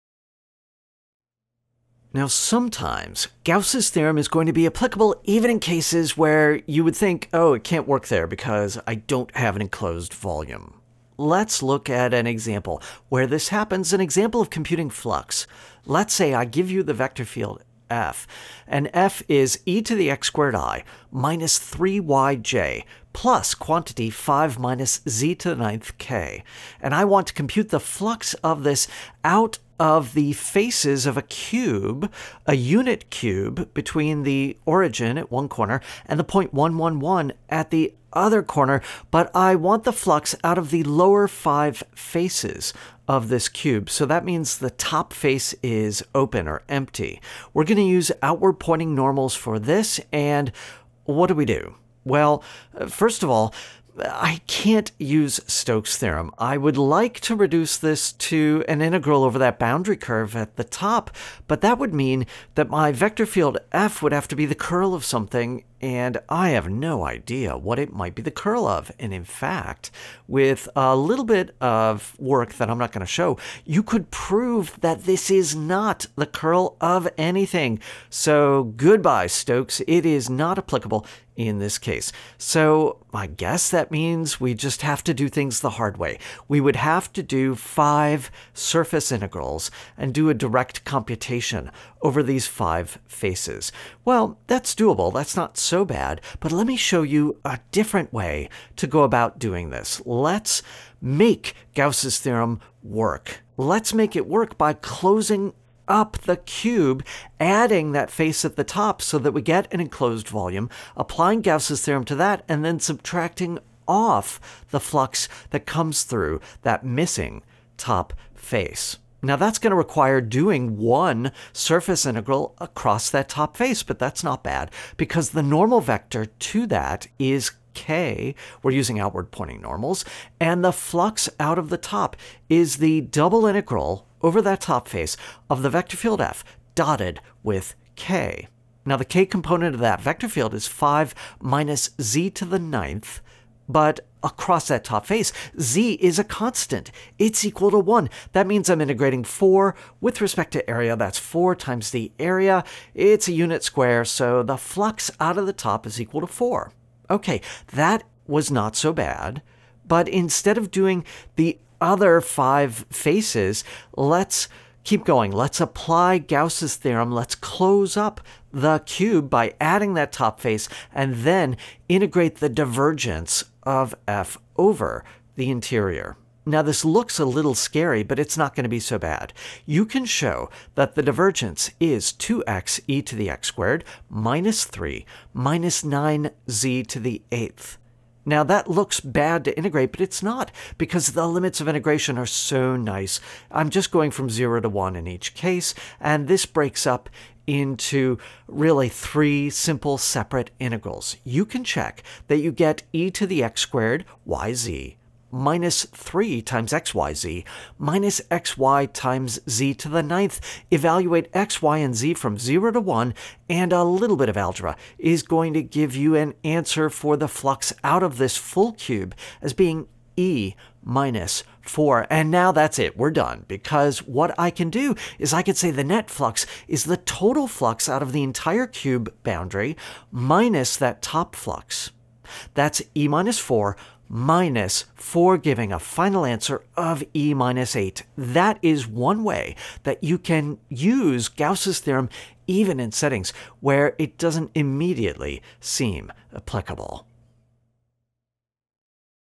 CalcBLUE 4 : Ch. 12.3 : Sneaky Example - Gauss's Theorem
Let's look at an example that does not *seem* to permit Gauss' Theorem, but for which the theorem is really useful.